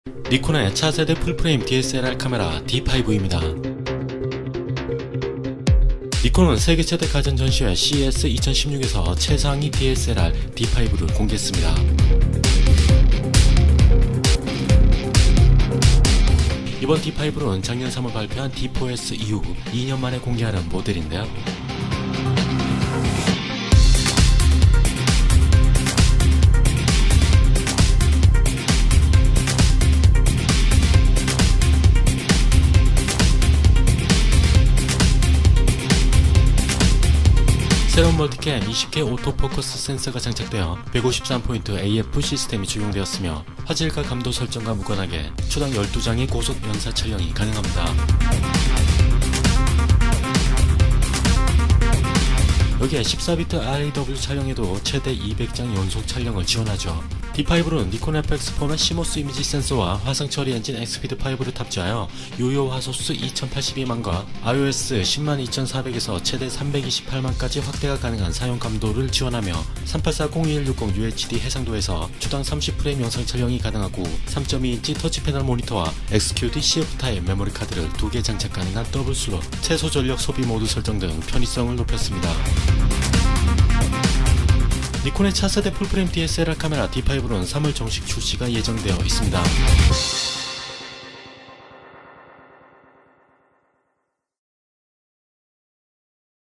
Nikon D5: 니콘의 차세대 풀 프레임 DSLR 카메라 'D5'-[스나이퍼 보도국]
니콘이 CES 2016에서 공개한 최상위 DSLR D5는 멀티 캠 20K 오토포커스 센서가 장착되어 153포인트 AF 시스템이 적용되었으며 화질 감도 설정과 무관하게 초당 12장의 고속 연사 촬영이 가능합니다.